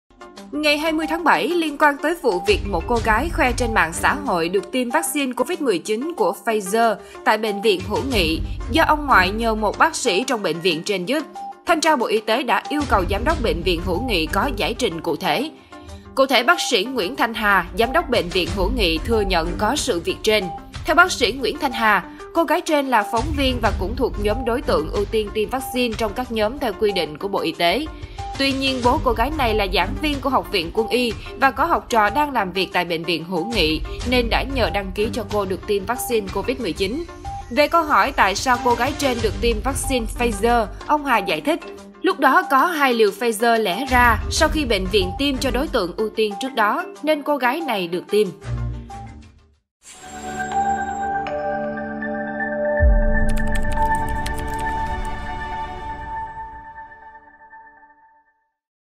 Trường hợp khoe tiêm vaccine Pfizer: Bộ Y tế yêu cầu Giám đốc Bệnh viện Hữu nghị giải trình
Ngày 20-7, liên quan tới vụ việc một cô gái khoe trên mạng xã hội được tiêm vaccine Covid-19 của Pfizer tại Bệnh viện Hữu nghị do “ông ngoại” nhờ một bác sĩ trong bệnh viện trên giúp, Thanh tra Bộ Y tế đã yêu cầu Giám đốc Bệnh viện Hữu nghị có giải trình cụ thể.